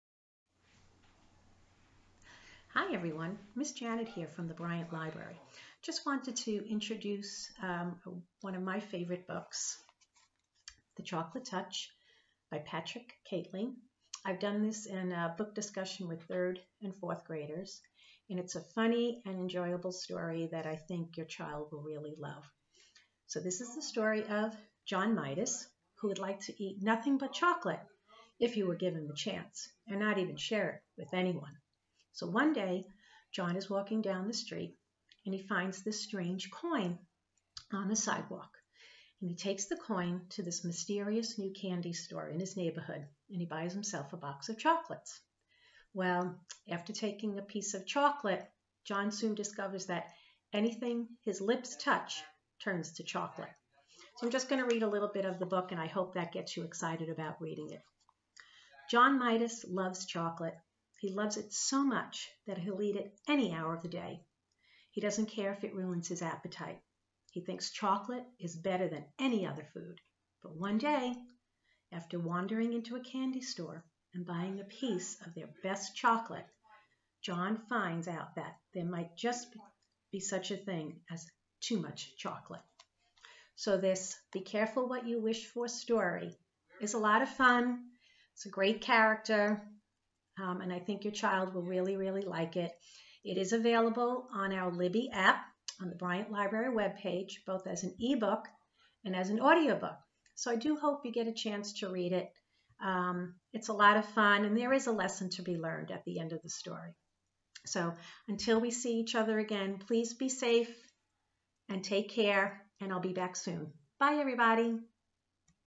The Chocolate Touch by Patrick Skene Catling - Children's Book Talks
Miss Janet talks about the book "The Chocolate Touch" by Patrick Skene Catling.